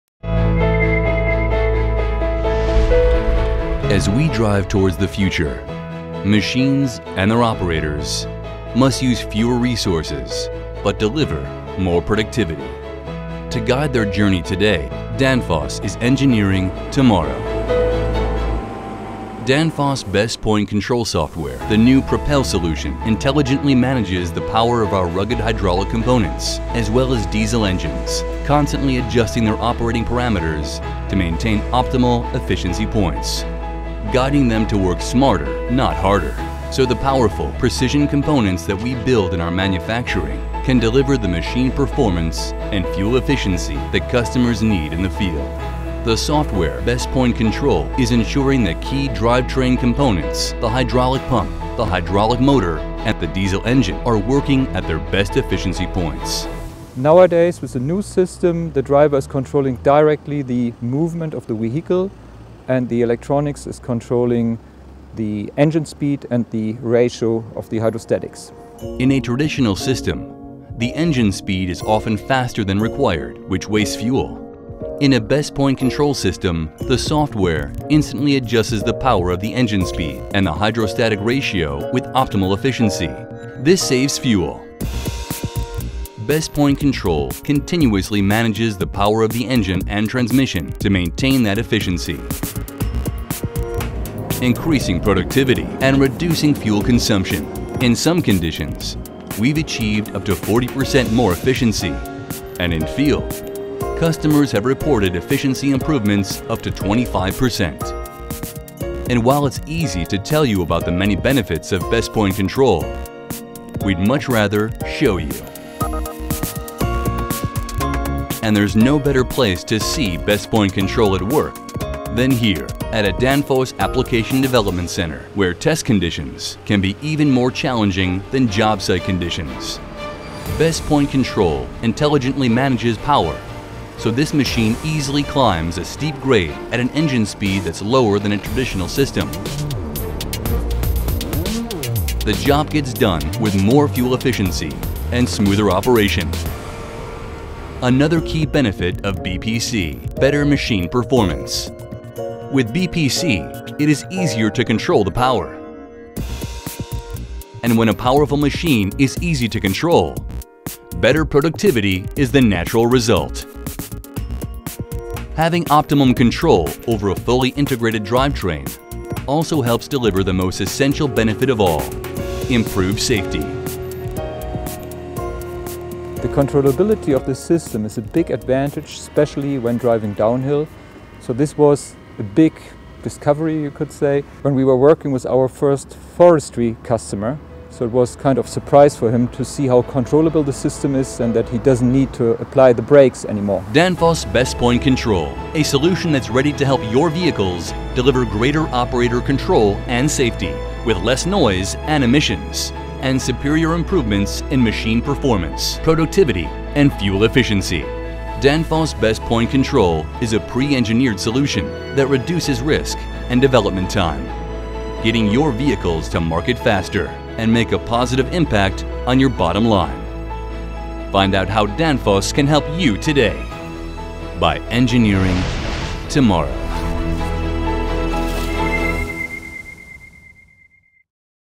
Best Point Control Video
Danfoss Power Solutions is pleased to announce the global launch of its Best Point Control (BPC) software technology, which increases machine efficiency levels by detecting the needed power for current operation conditions and automatically adjusting the engine speed to meet optimum efficiency points.